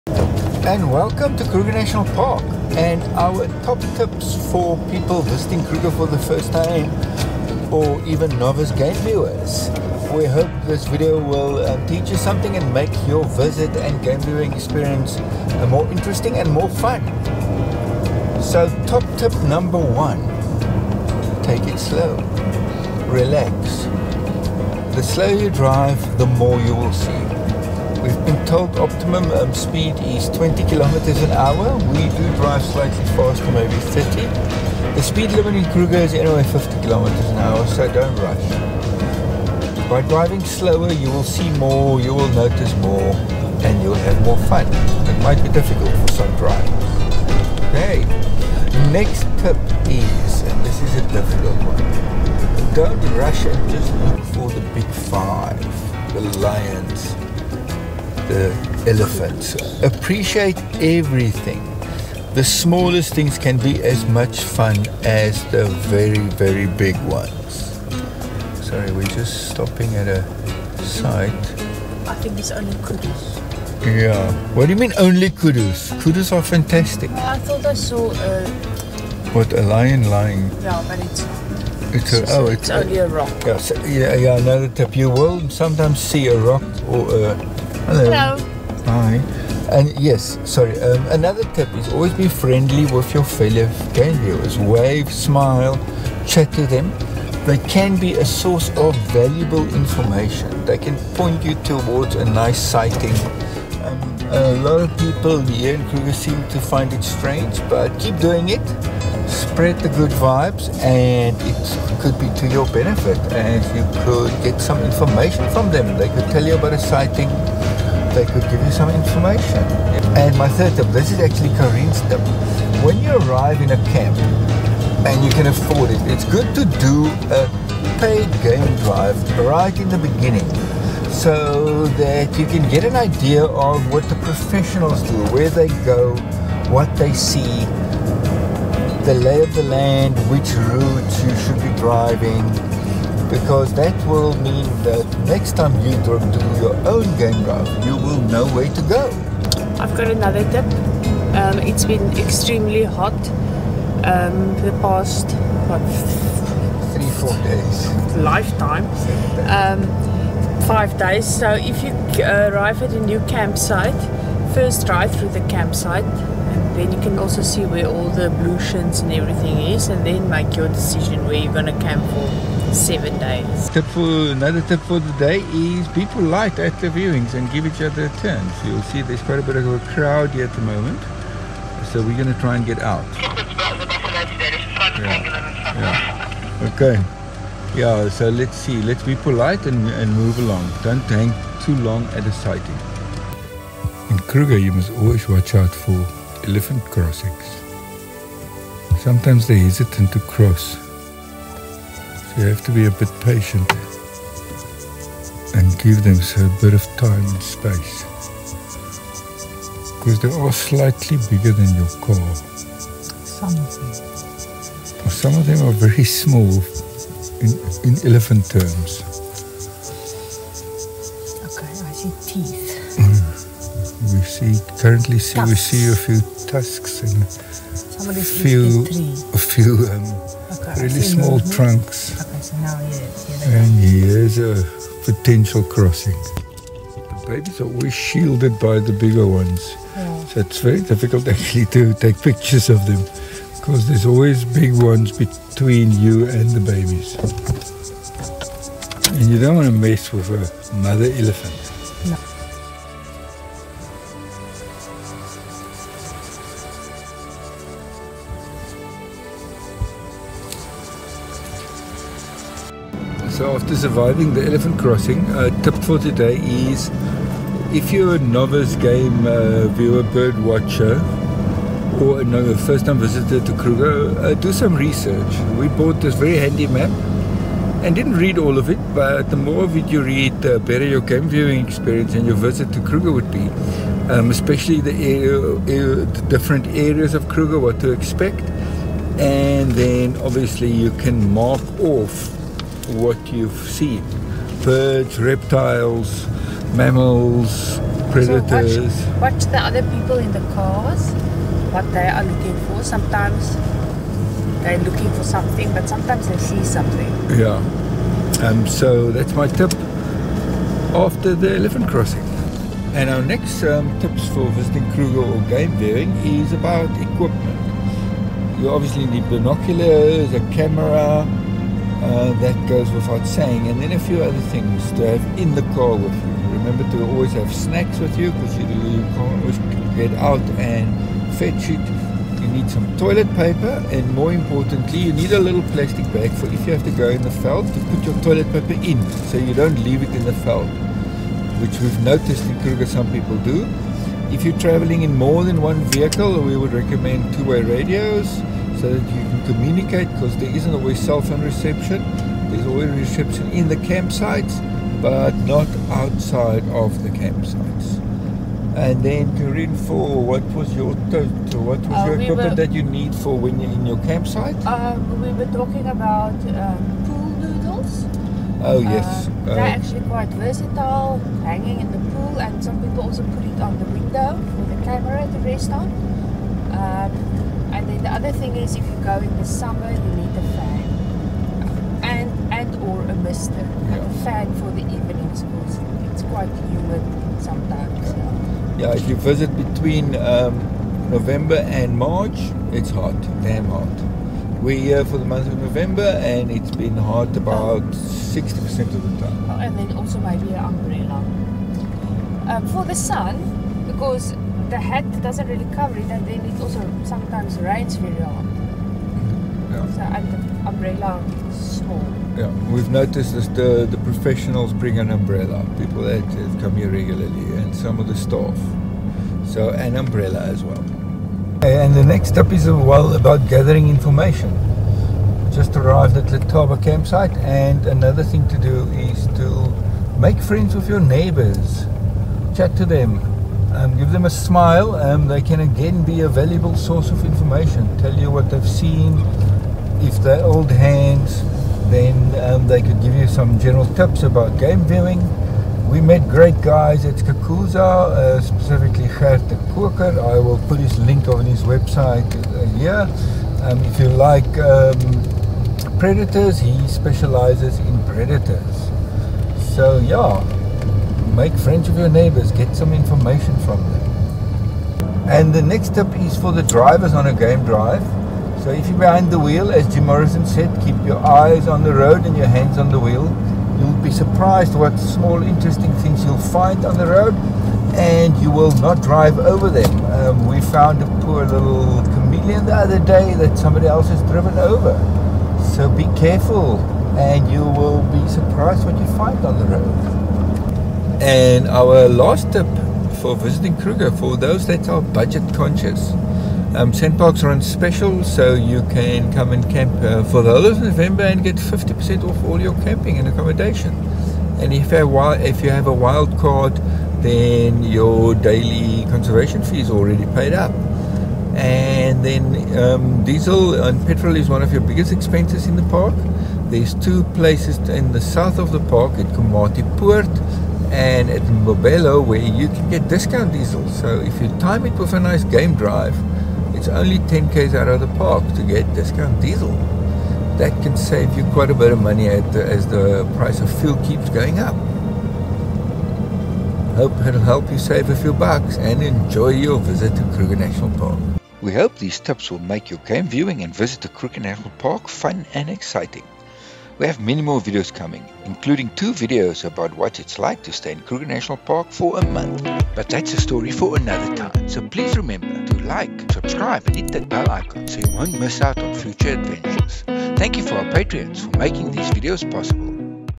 Self-drive safari in Kruger National Park Tips
Here we share our tips and tricks for visiting Kruger National Park. If you are planning a self-drive safari and game viewing vacation then this video is for you. We share our experience in visiting Kruger National Park. What equipment you should bring and how to get the best game viewing and self driving Safari experience.

Thank you to our Patreons for making this video possible. If you want to support us then go